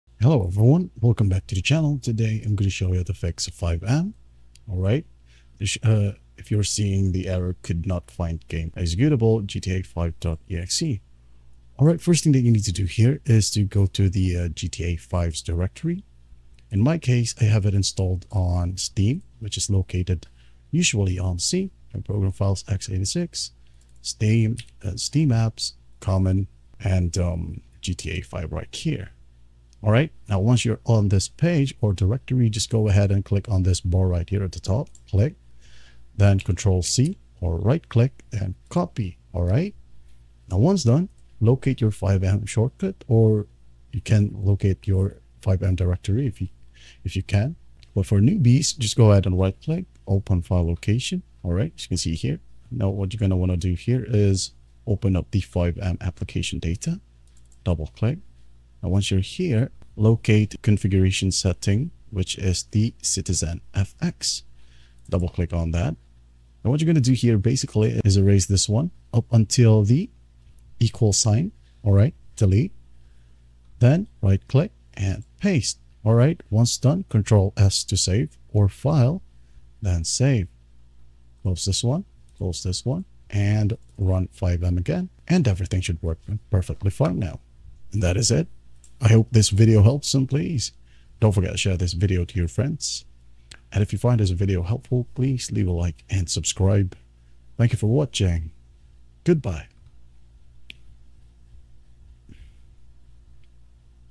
How To Fix FiveM Could Not Find Game Executable (GTA5.exe) - Full Guide 2025
Welcome in this video I will show you How To Fix FiveM Could Not Find Game Executable (GTA5.exe).